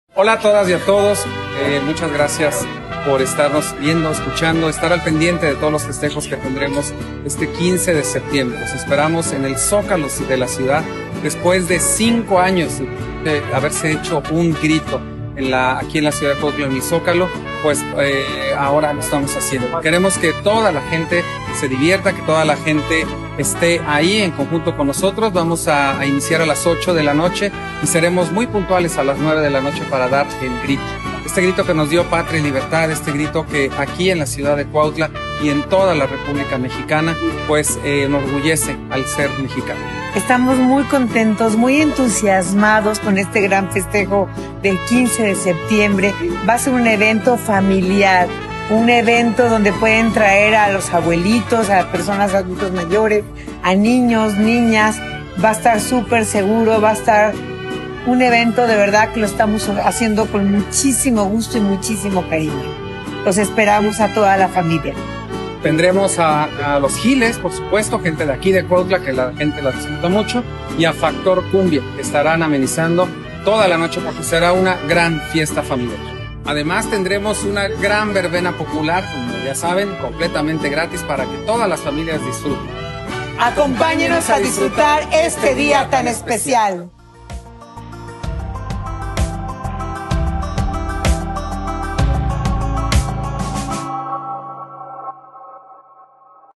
INVITACIÓN A LOS FESTEJOS DE LAS FIESTAS PATRIAS A TODOS LOS CUAUTLENSES.
El Presidente Municipal de Cuautla, Rodrigo Arredondo y la titular del SMDIF, Araceli García Garnica, te invitan a celebrar el Grito de Independencia este 15 de septiembre a las 8:00 pm en el Zócalo de Cuautla.